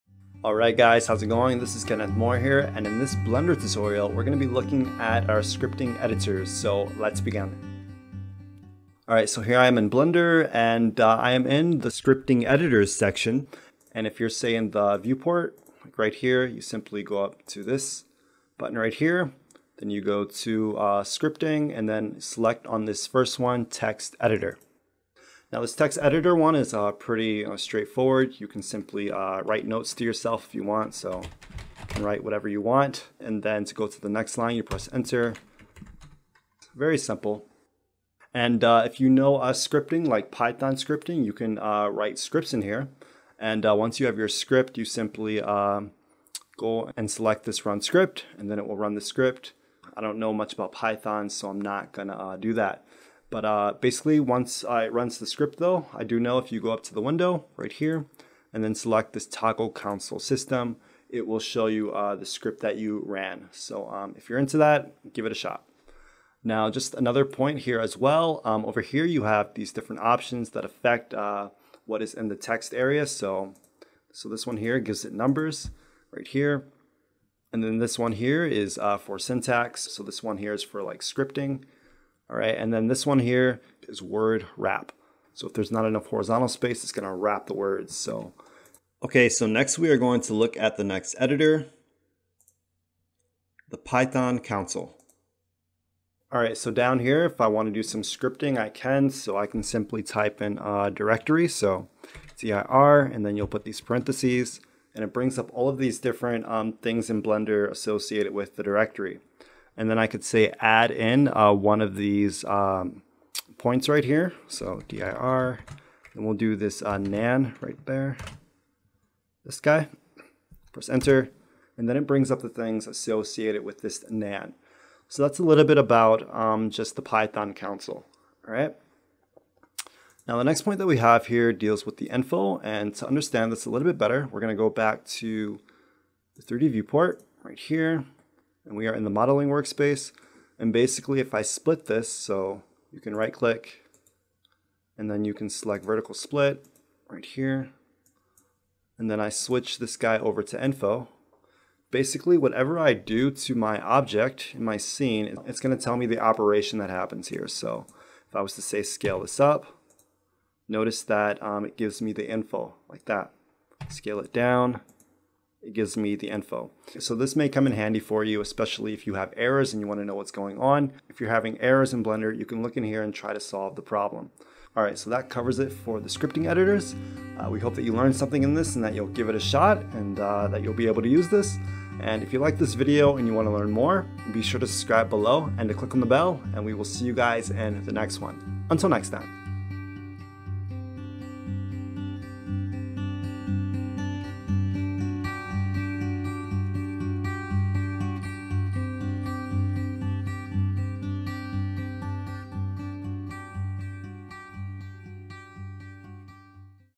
Scripting Editors - Blender Tutorial
In this video we cover the Scripting Editors in Blender